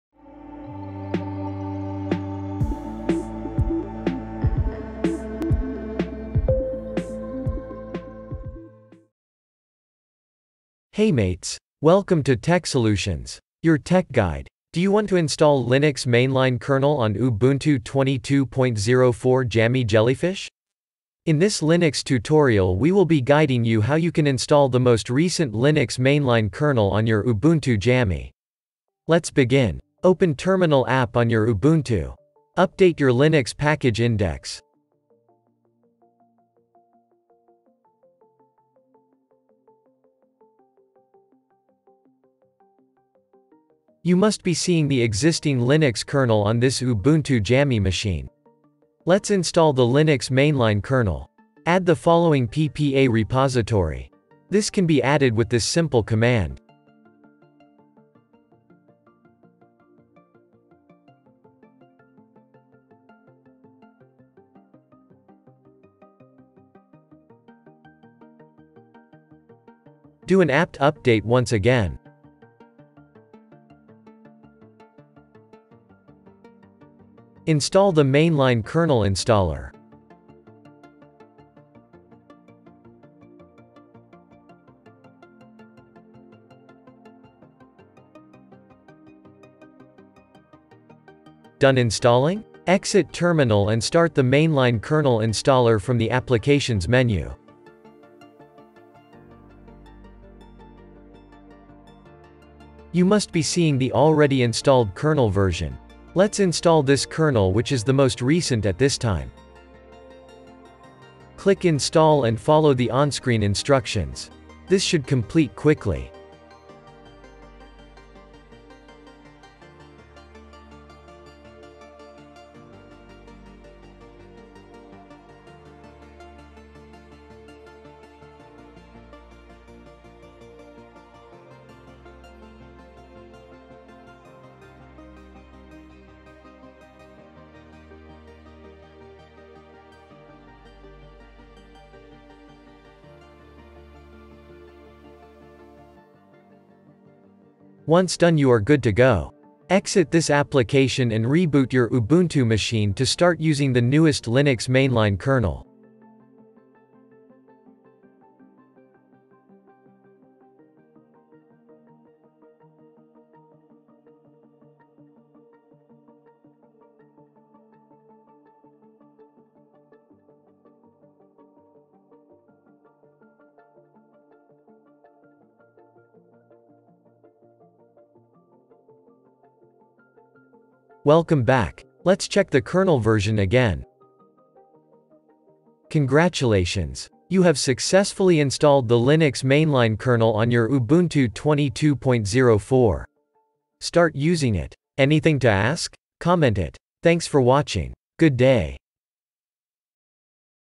How to Install Linux Kernel [ Mainline ] on Ubuntu 22.04 Linux Kernel Customizations 2022
How to Install Linux Kernel [ Mainline ] on Ubuntu 22.04 Linux Kernel Customizations 2022 Customization Linux Kernel Ubuntu users, especially LTS users, are well aware that the current shipped kernel focuses on stability over bleeding-edge Linux mainline kernel releases. This is an excellent method for those seeking a stable, uninterrupted environment, but you may often have new hardware that requires a more recent kernel with support for it. An excellent option for Ubuntu desktop users to solve this is the Ubuntu Mainline Kernel Installer, which features a graphical UI that provides the option to install the latest kernel and other versions if you are after a particular version.